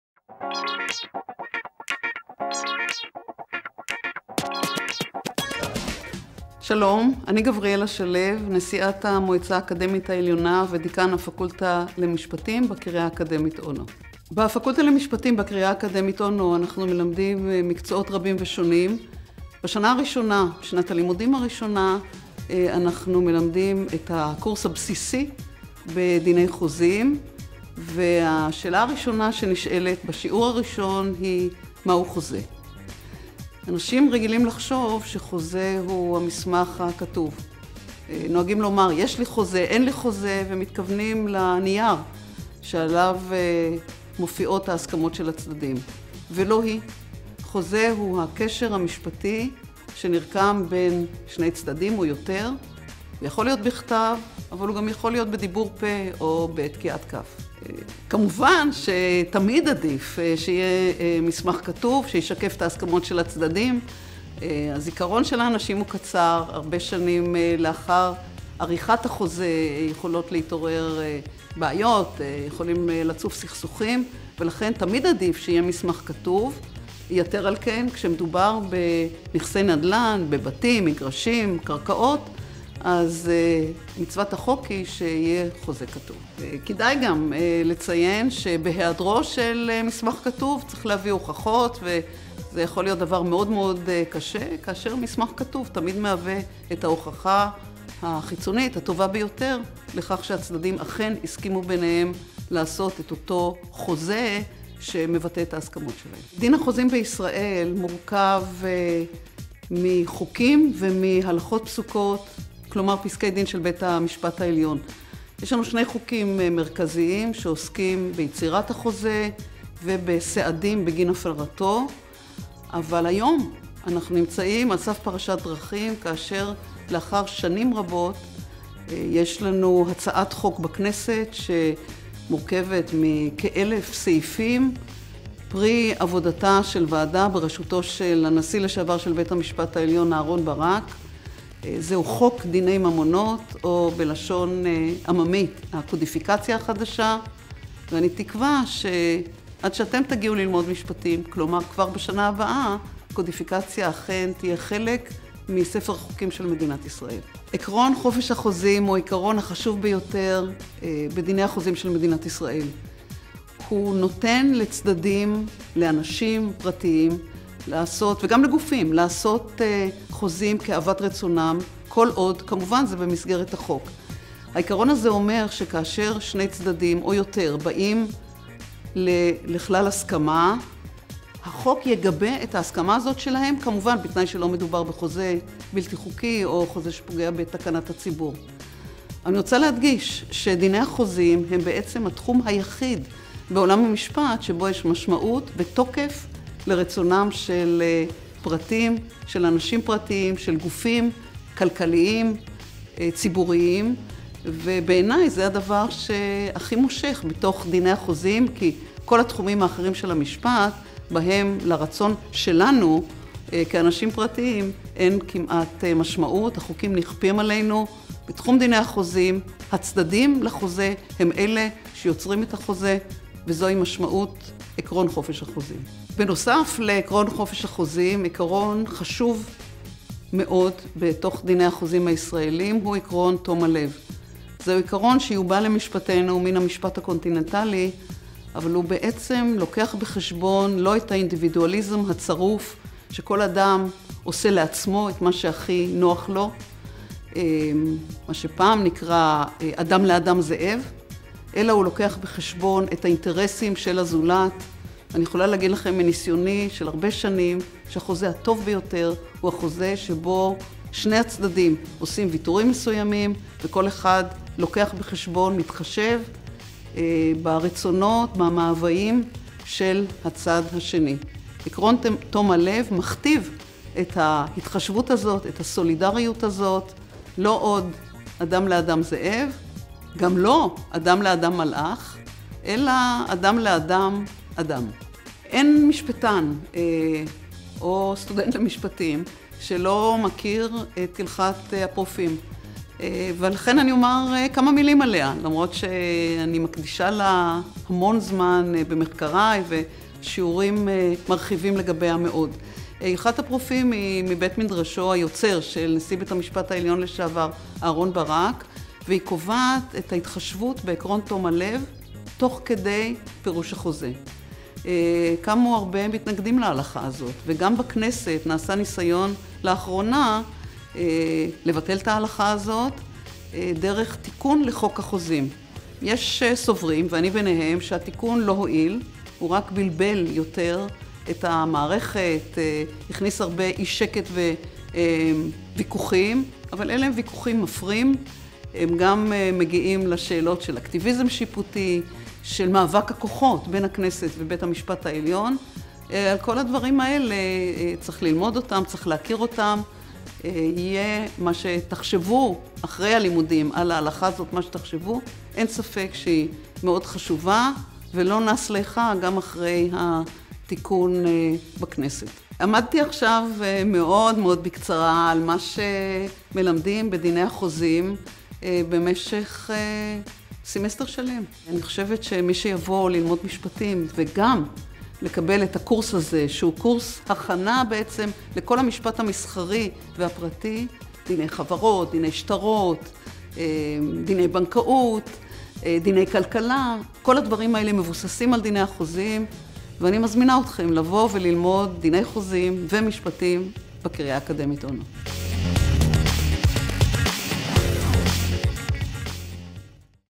פרופ' גבריאלה שלו על דיני חוזים
פרופ' גבריאלה שלו, לשעבר שגרירת ישראל באו"ם ודיקן הפקולטה למשפטים על דיני חוזים